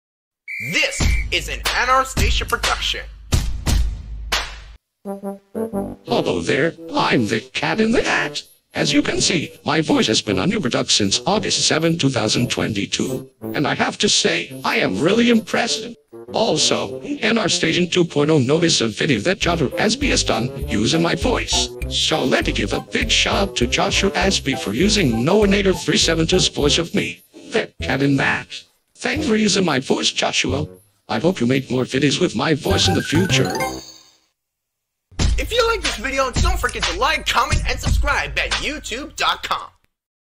A big shoutout to Joshua Ashby!!!
Since I just saw Joshua Ashby's two videos using The Cat in the Hat's Uberduck voice, I decided to make this video just for him.